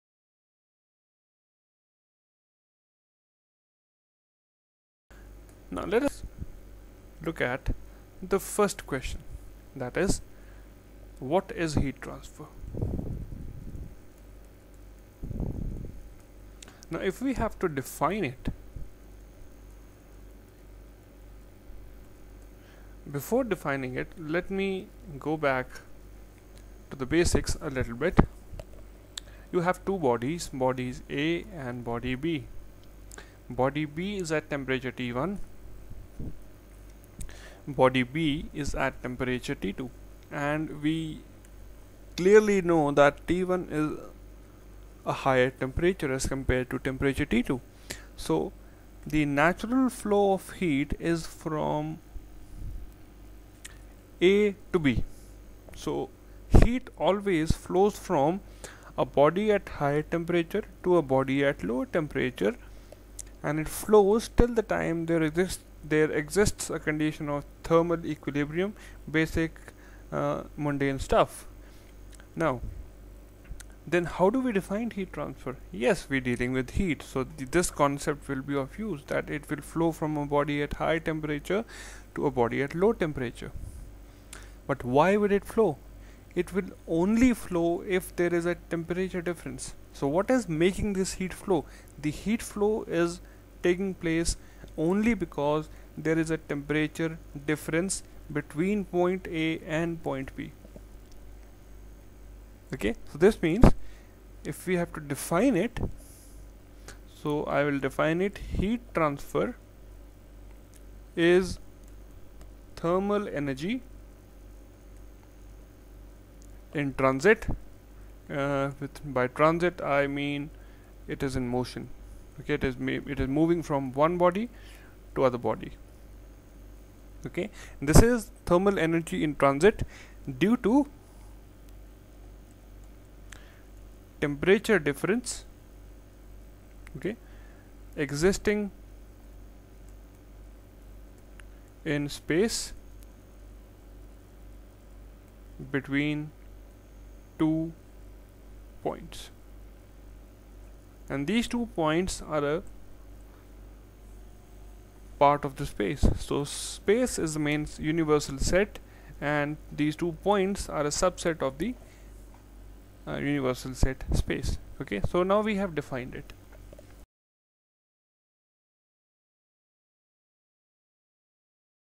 Define Heat Transfer - What is heat transfer- Heat Transfer - GATE Mechanical
This video will define Heat Transfer. Learn what is Heat transfer. Learn the concept of Heat Transfer. Heat transfer basics. It is a sub-topic under the Heat Transfer Section in GATE Mechanical Exam.